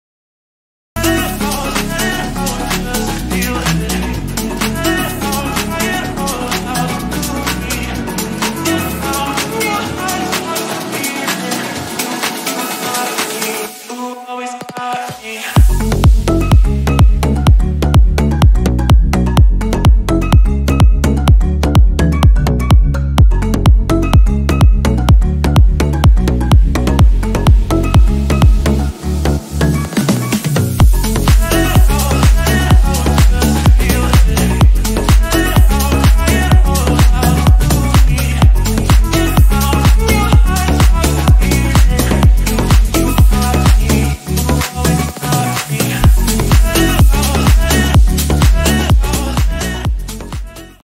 Rally Driving at Cotswold Driving Experiences
A quick snippet to show you what to expect from one of our award winning Rally Driving Experiences